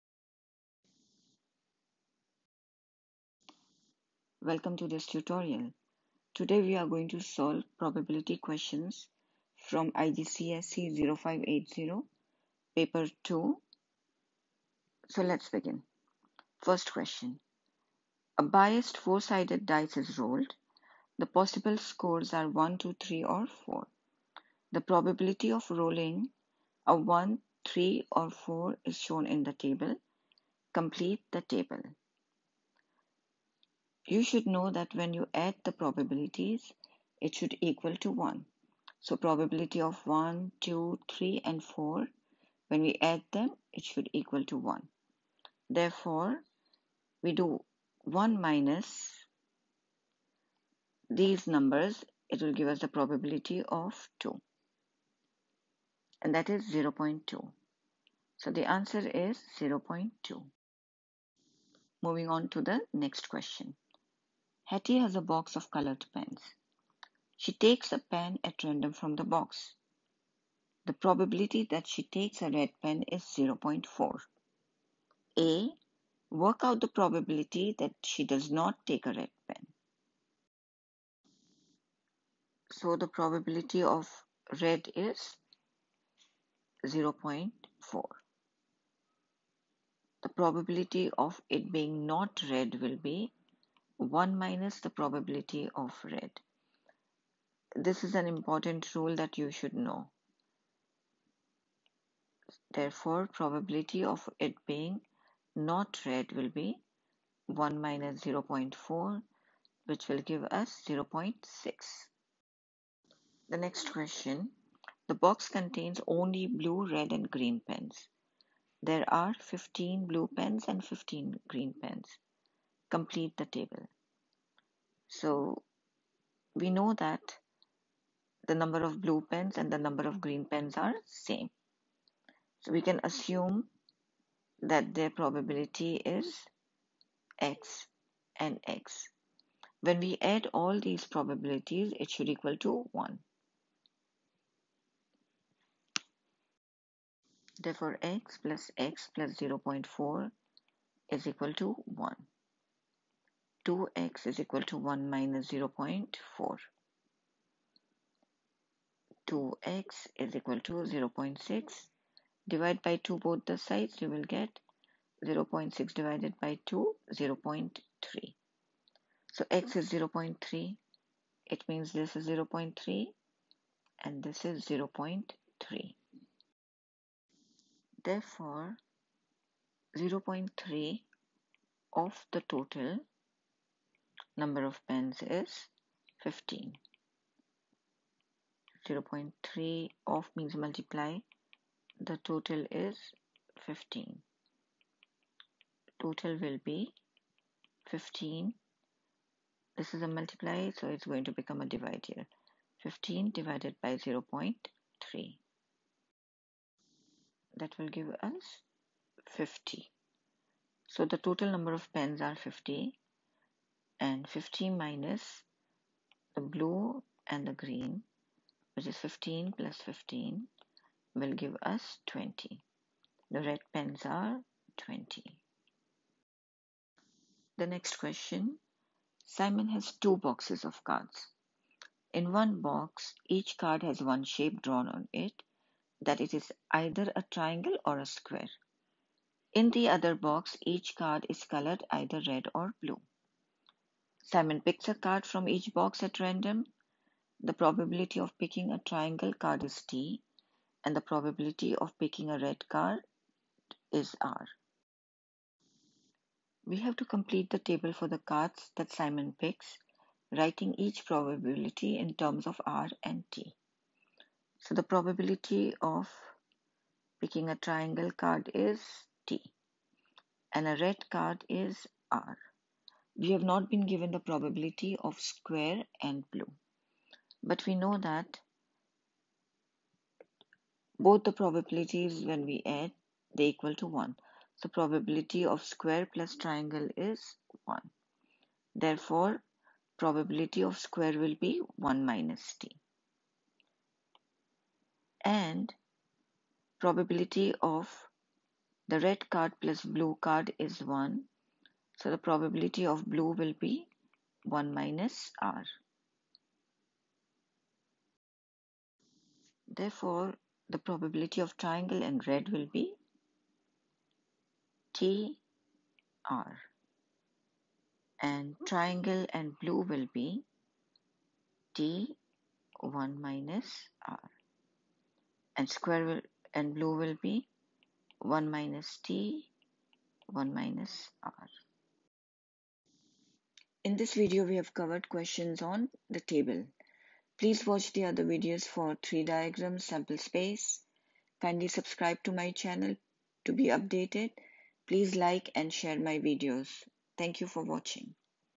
IGCSE 0580 PAST PAPER QUESTIONS PROBABILITY TABLE 2A
IN THIS VIDEO WE SOLVE QUESTIONS FROM IGCSE 0580 PAST PAPER 2. YOU CAN PRACTICE THEM FOR PAPER 1, 3 AND 4 AS WELL AND FOR GCSE. THE BEST WAY TO SCORE MARKS IS TO SOLVE PAST PAPER QUESTIONS TOPICWISE.